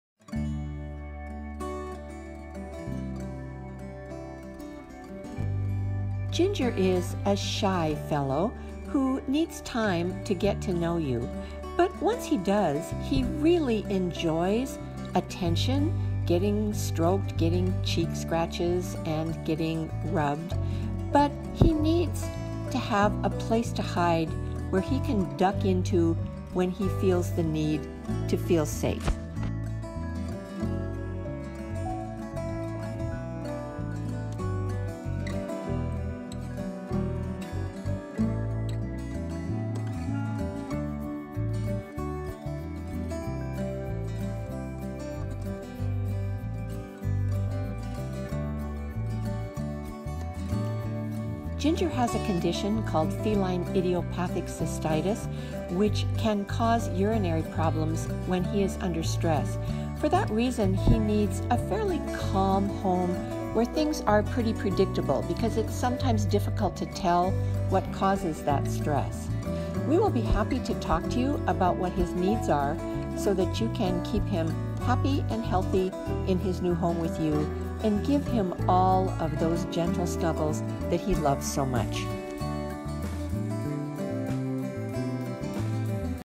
Meet Ginger! (#246484)
What my friends at OHS say about me:
Need some biscuits with your breakfast? I’m an expert kneader!
Cheek and chin scratches make me so happy!
Fear and Anxiety: I can be worried about new people, new surroundings and touch. I prefer to take things at my own pace and will need to go home to a calm environment.
House Training: I have a history of house training struggles which could be related to my medical condition. I have a medical condition called feline idiopathic cystitis (FIC) and will need to live in a calm environment. This will require lifelong management.
I have been diagnosed with dental disease which means I may need dental cleanings or extractions in the future.

The type of home I’m looking for:
I am declawed and will need to live indoors only!